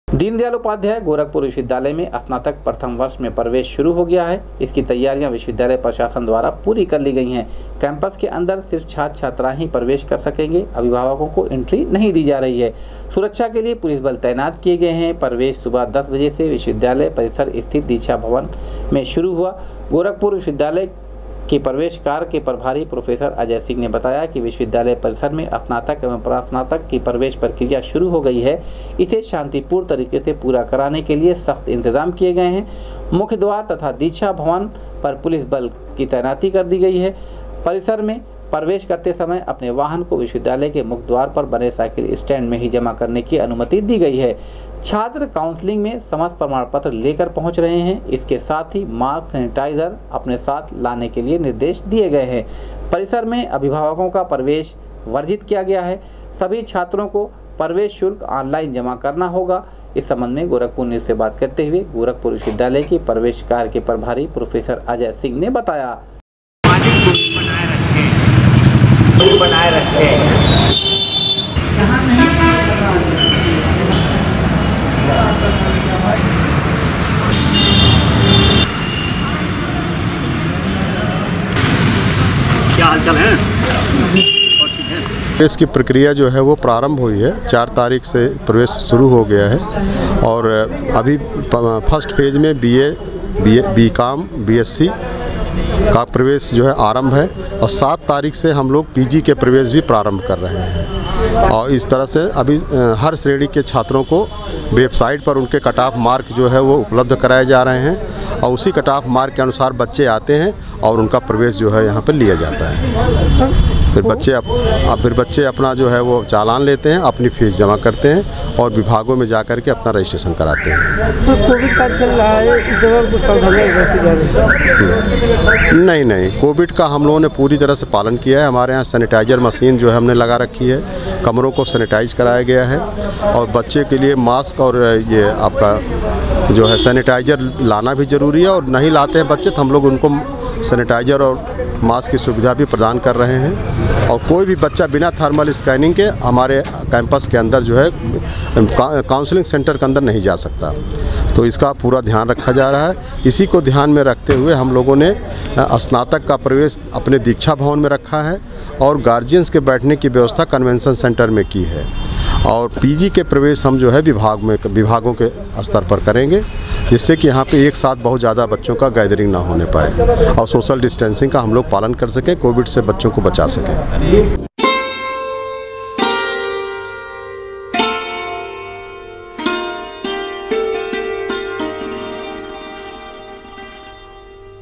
गोविवि में स्नातक एवं परास्नातक की प्रवेश प्रक्रिया हुई शुरू, काउंसलिंग में समस्त प्रमाण पत्र लेकर
गोविवि में स्नातक एवं परास्नातक की प्रवेश प्रक्रिया हुई शुरू, काउंसलिंग में समस्त प्रमाण पत्र लेकर पहुंच रहे छात्र छात्राएं, विश्वविद्यालय परिसर में परिजनों का प्रवेश है वर्जित